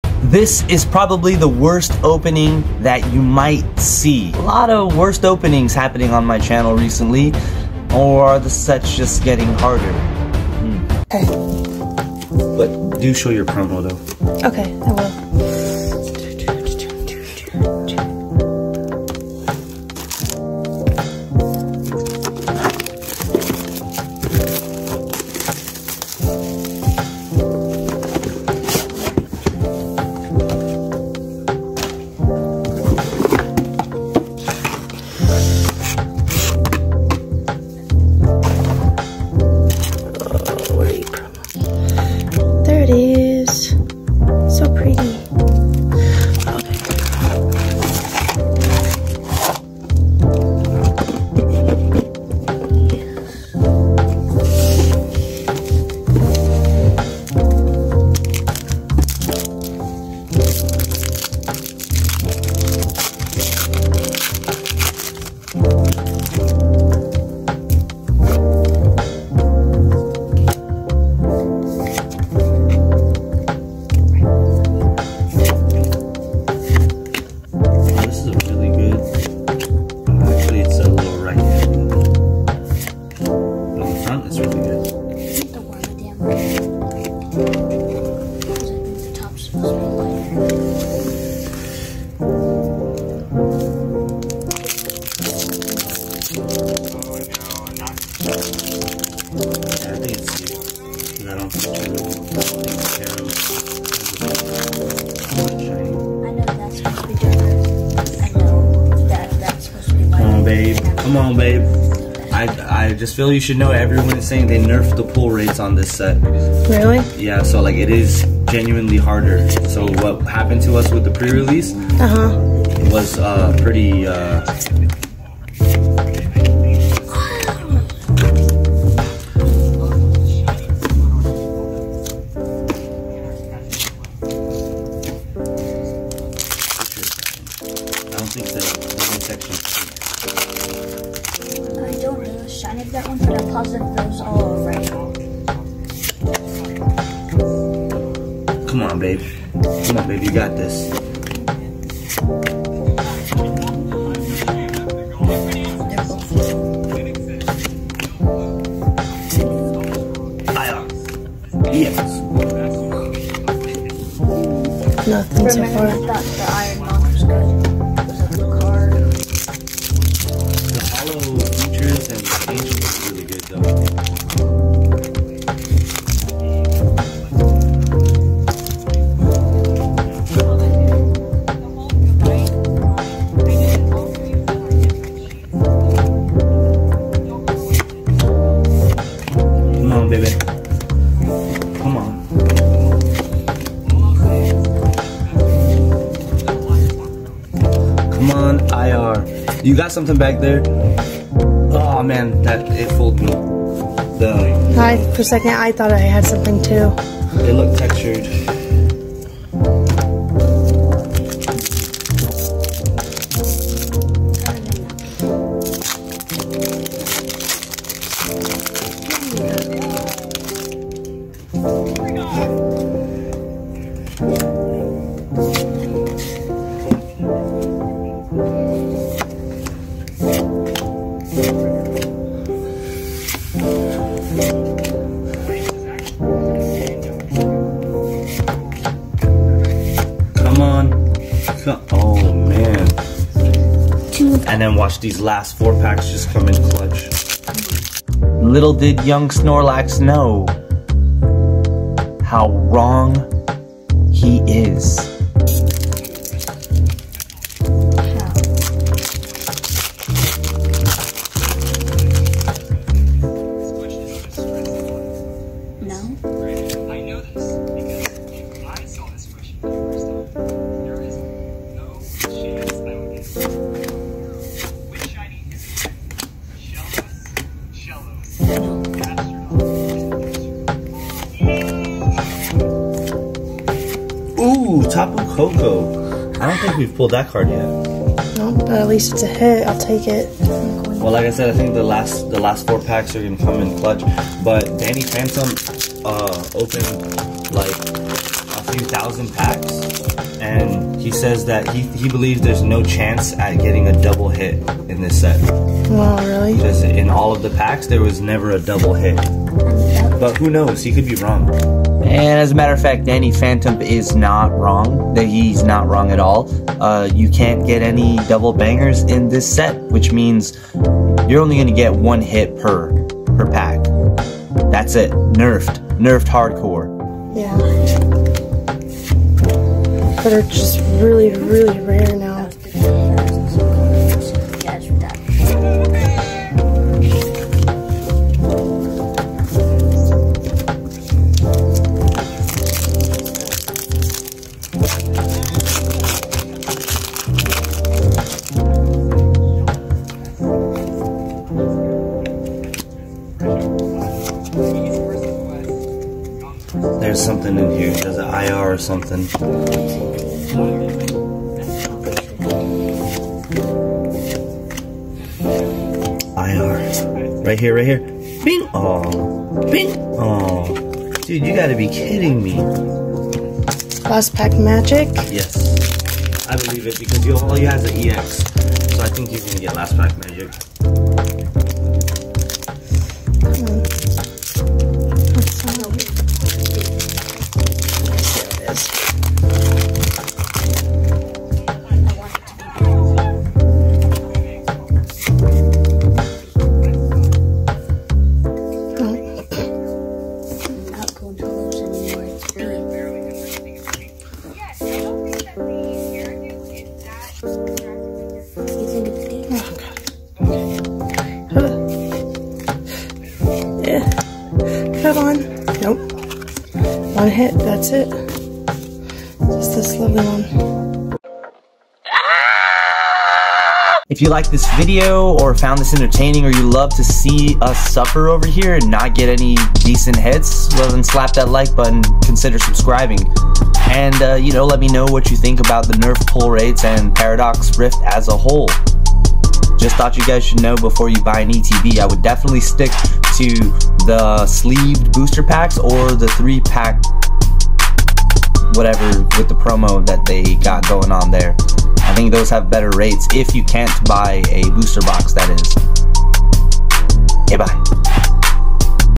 INSANELY BAD OPENING
I've known ETB's are a hit or miss but this was CRAZY! When I was told they nerfed the pull rates, I didn't think it was this bad. Or, did I just get a dud box?